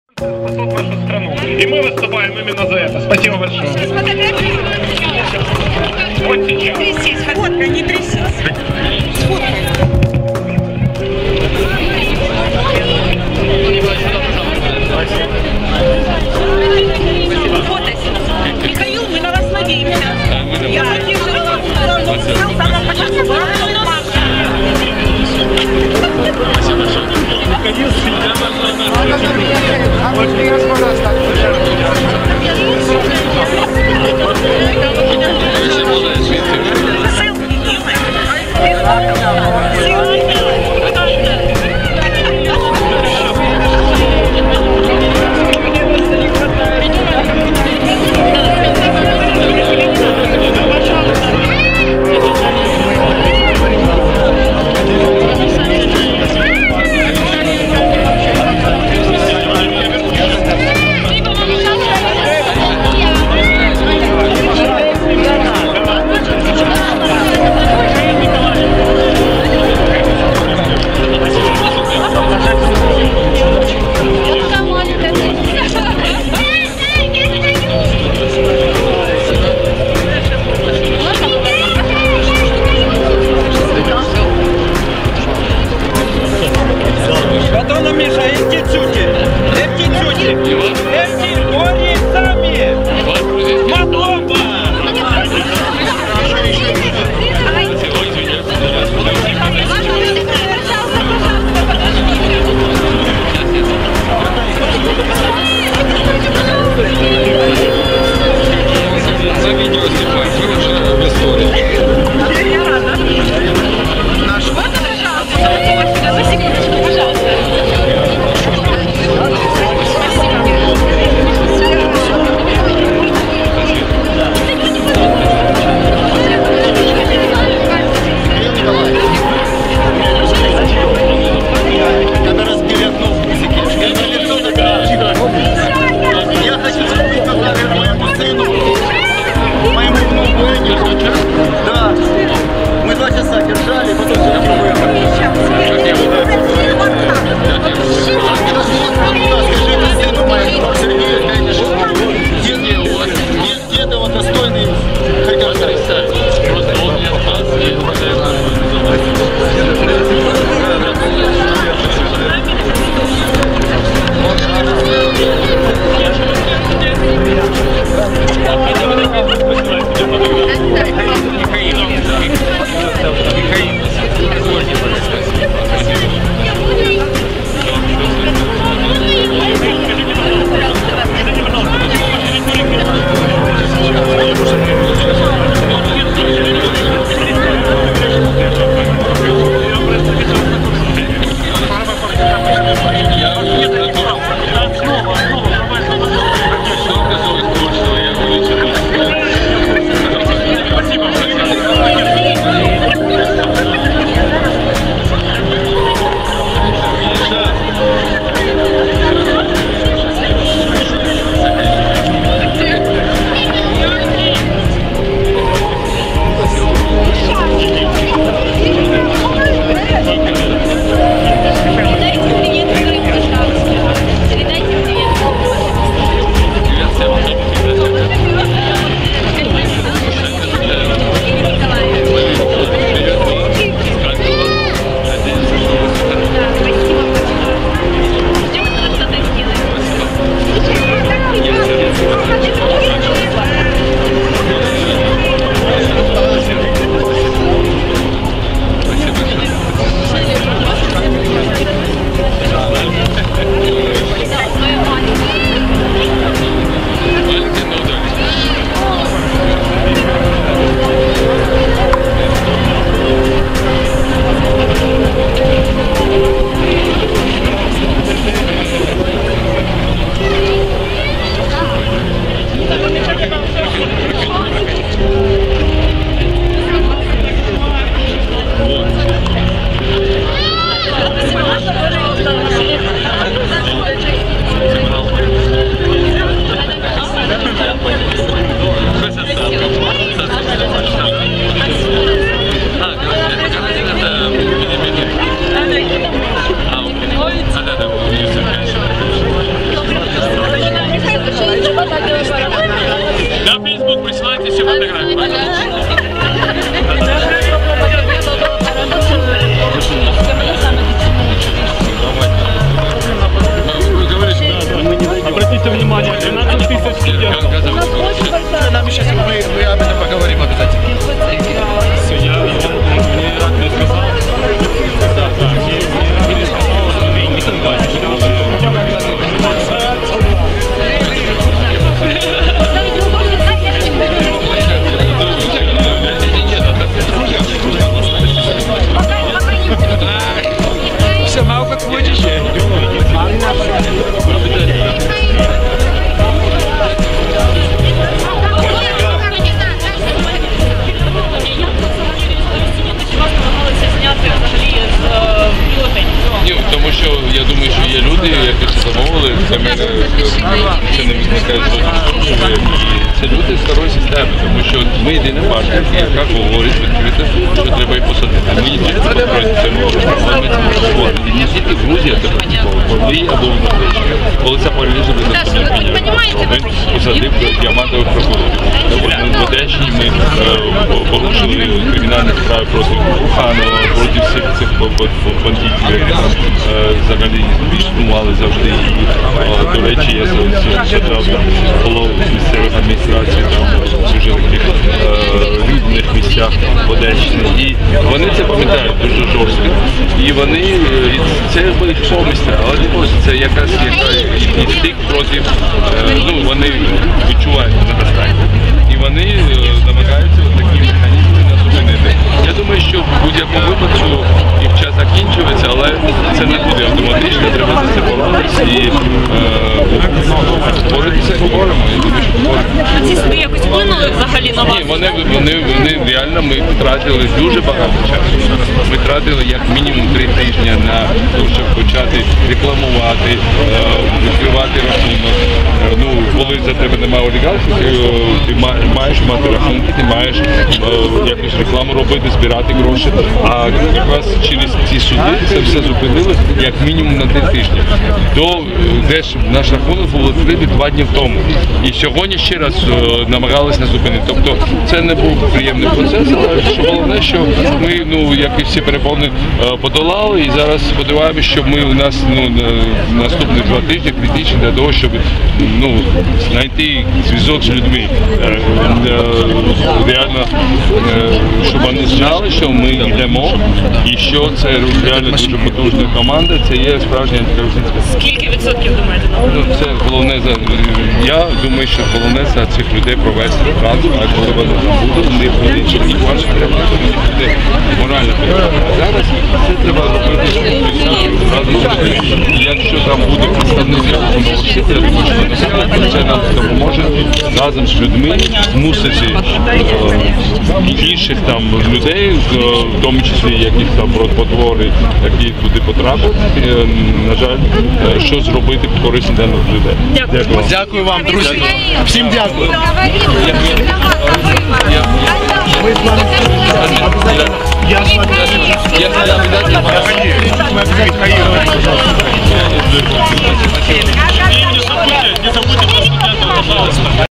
Сэлферы Саакашвілі в Харкові + інтерв'ю 2019
Сэлферы Саакашвілі в Харкові і інтервью о проблемах регістрації партії Рух Нових Сил - Харків 2019